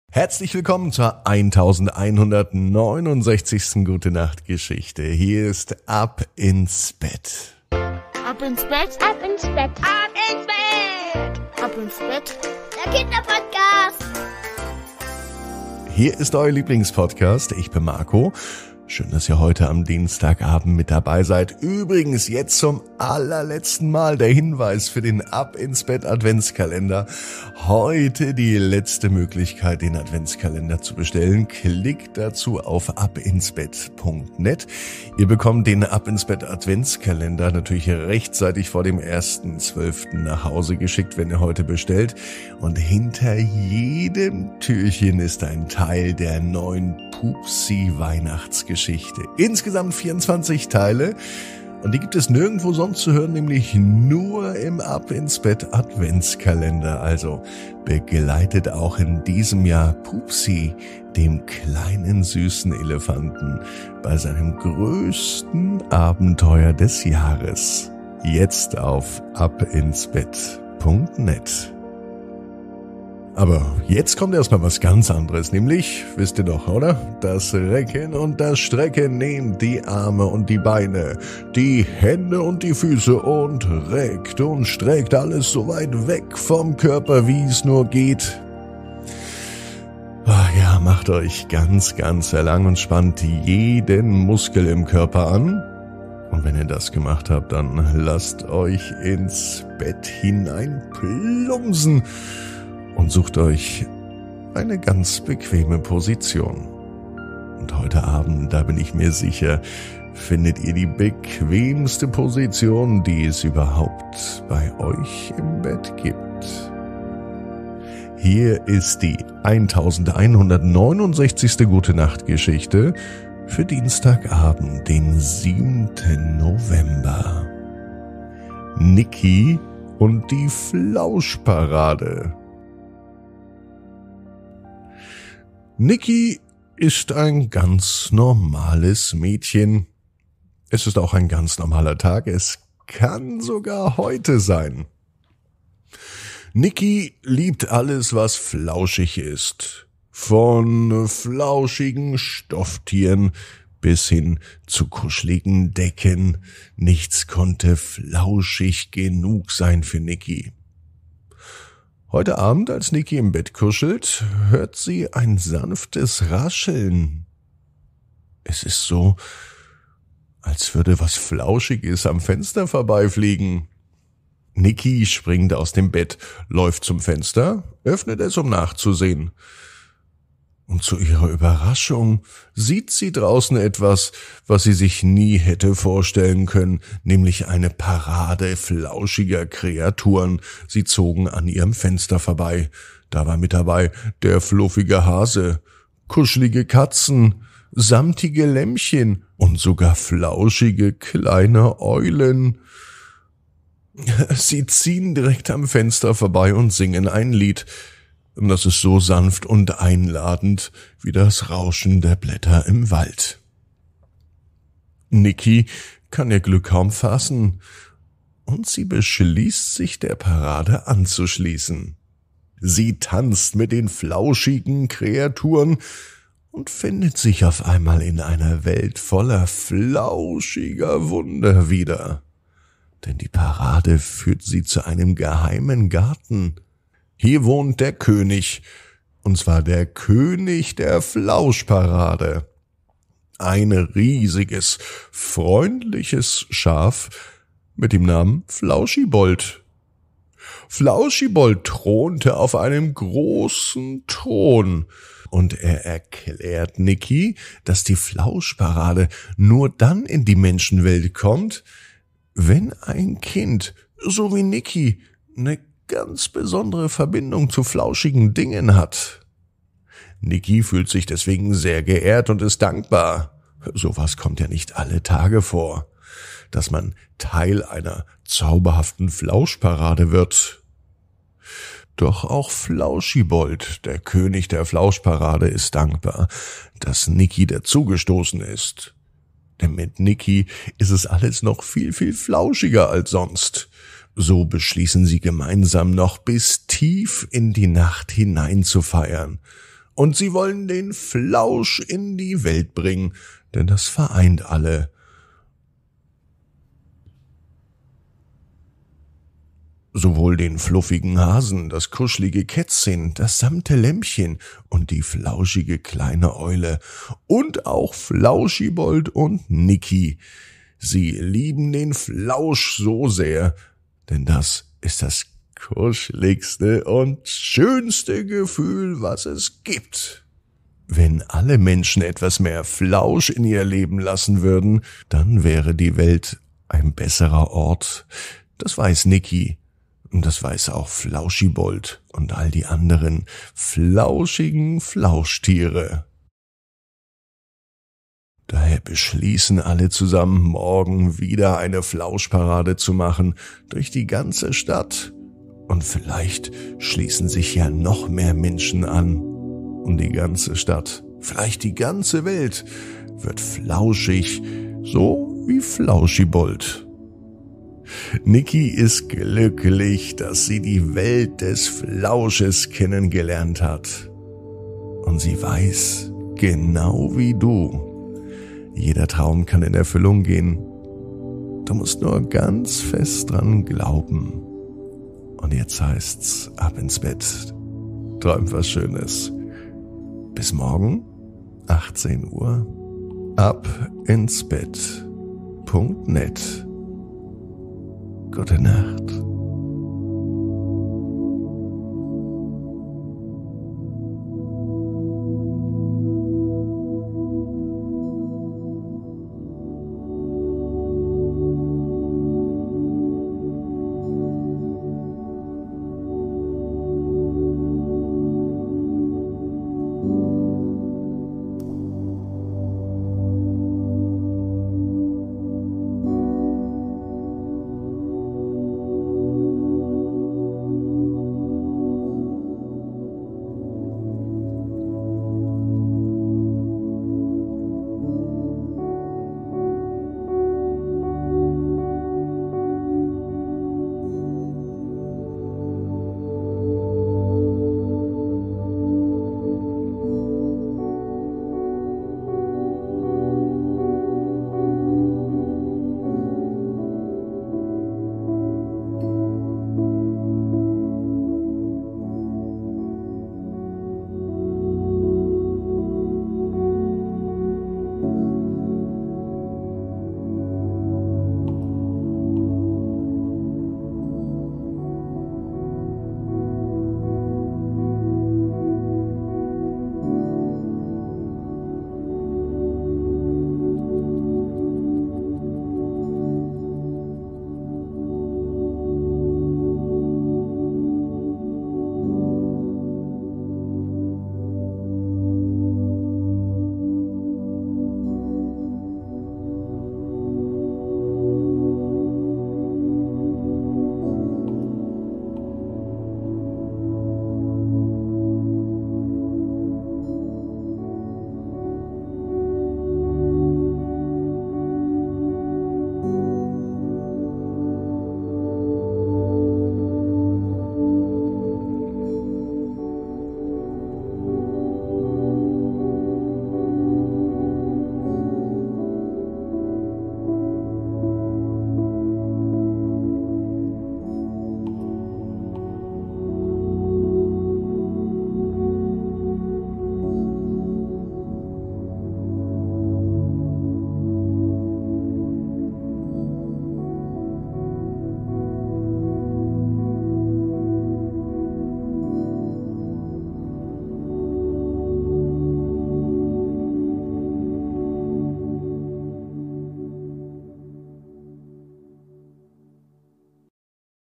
#1169 Nicki und die Flauschparade - Ab ins Bett - Die tägliche Gute Nacht Geschichte
In dieser Gute-Nacht-Geschichte begleiten wir Nicki, ein Kind mit einer besonderen Vorliebe für alles Flauschige. Doch eines Abends erlebt sie ein unglaubliches Abenteuer, als eine Parade der flauschigsten Kreaturen vor seinem Fenster auftaucht. Tauche ein in eine Welt voller flauschiger Wunder und lass dich von Nickis Freude anstecken, während sie den König der Flauschparade trifft.